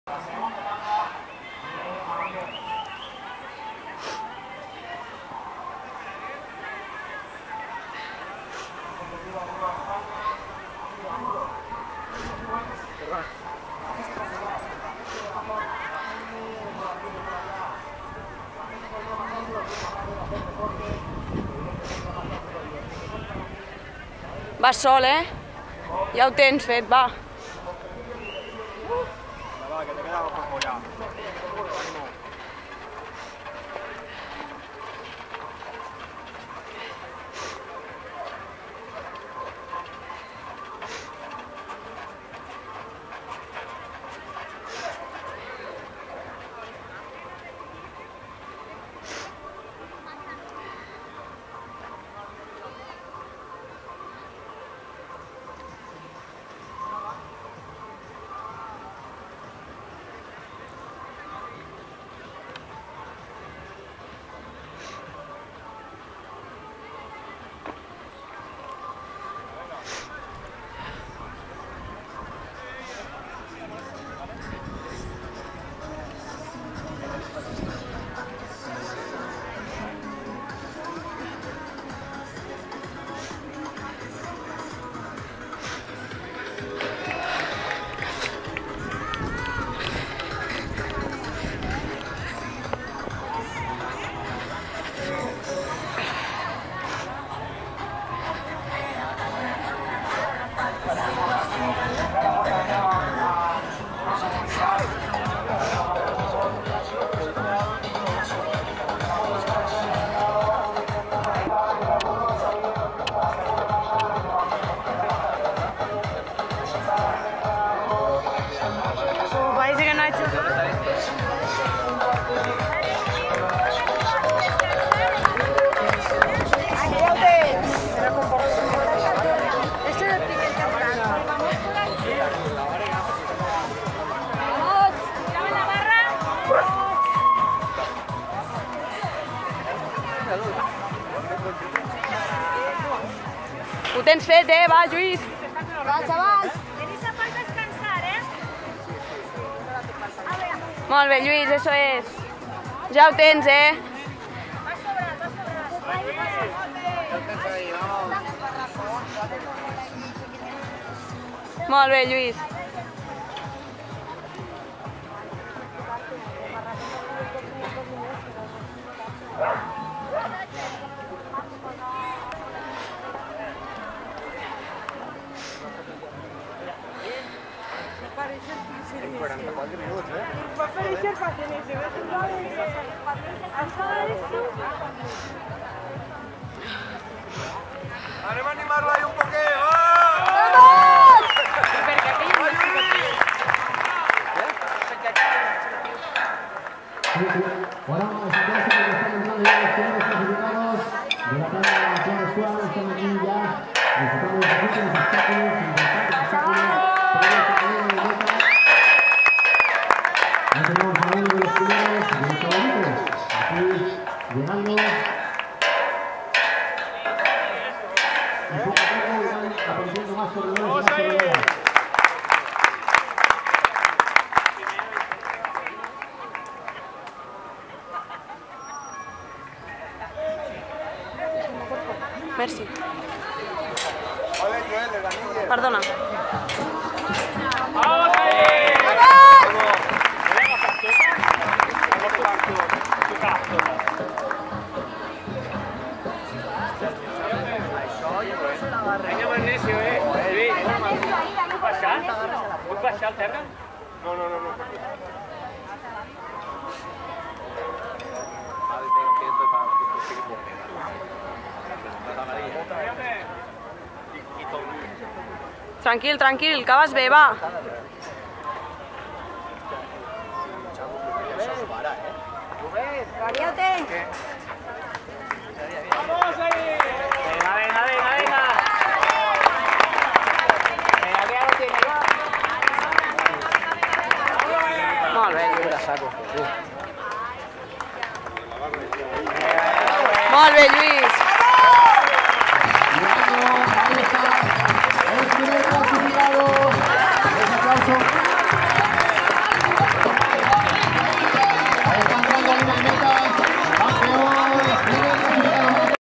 FINAL LLUIS BARBÉ EGYPTIAN 2017
Final del circuito de la Egyptian donde logro ganar la a prueba.